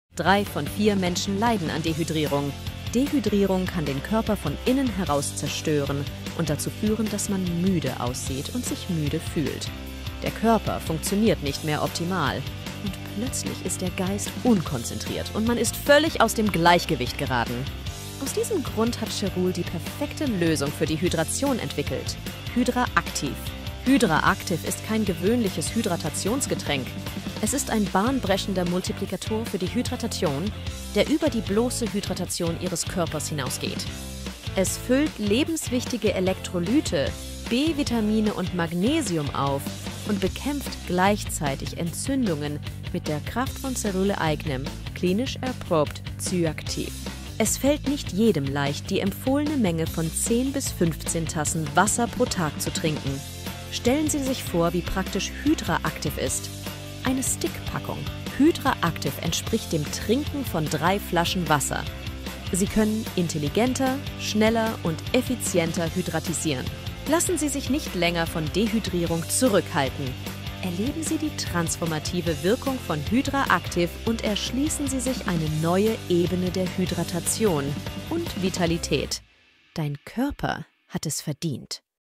HydraActiv ERHÖHT DIE HYDRATION | REDUZIERT ENTZÜNDUNGEN
Versorgen Sie Ihren Körper mit Flüssigkeit wie nie zuvor. Wir präsentieren HydraActiv: Cerules Hydratationsmultiplikator, angereichert mit Cyactiv und Magnesiumglycinat.

HydraActiv wird durch eine einzigartige Mischung sorgfältig ausgewählter Inhaltsstoffe angetrieben, um die Leistung Ihres Körpers zu optimieren. Lassen Sie uns einen Blick auf die Schlüsselkomponenten werfen, die HydraActiv so außergewöhnlich machen:

➡️ CYACTIV: ein patentierter Extrakt aus Spirulina. Cyactiv unterstützt eine gesunde Entzündungsreaktion im Körper und fördert das allgemeine Wohlbefinden und die Vitalität.

➡️ MAGNESIUMGLYCINAT: ein Mineral, das für seine wichtige Rolle bei verschiedenen Körperfunktionen bekannt ist. HydraActiv nutzt die Kraft von Magnesium, um Muskelentspannung und -funktion zu unterstützen, gesunde Energieniveaus zu fördern, beim Stressmanagement zu helfen und gesunde Leber- und Herz-Kreislauf-Funktionen zu unterstützen.

➡️ B-VITAMINE: ein weiteres entscheidendes Element in der Formel von HydraActiv. Diese wasserlöslichen Vitamine spielen eine wichtige Rolle bei der Aufrechterhaltung einer optimalen Zellfunktion und unterstützen die natürlichen Energieproduktionsprozesse Ihres Körpers. B-Vitamine sollen Ihnen helfen, sich den ganzen Tag über energiegeladener zu fühlen.

➡️ ELEKROLYTE: HydraActiv ist mit Elektrolyten angereichert, die wichtige Mineralien ersetzen, die durch Schweiß oder körperliche Anstrengung verloren gehen. Diese Elektrolyte helfen dabei, den richtigen Flüssigkeitshaushalt aufrechtzuerhalten, die Muskelfunktion zu unterstützen und zur allgemeinen Erholung nach dem Training oder Phasen intensiver Aktivität beizutragen.

Egal, ob Sie ein Sportler sind, der nach optimaler Leistung sucht, oder einfach nur Ihren Alltag unterstützen möchten, Sie können eine wirksame Kombination dieser Inhaltsstoffe erleben, die harmonisch zusammenwirken, um Ihr allgemeines Wohlbefinden zu steigern.

Verpassen Sie nicht, herauszufinden, wie HydraActiv Ihre Wellness-Routine revolutionieren kann.

* Diese Aussagen wurden nicht von der Food and Drug Administration bewertet. Dieses Produkt ist nicht zur Diagnose, Behandlung, Heilung oder Vorbeugung von Krankheiten bestimmt.

Möchten Sie mehr erfahren?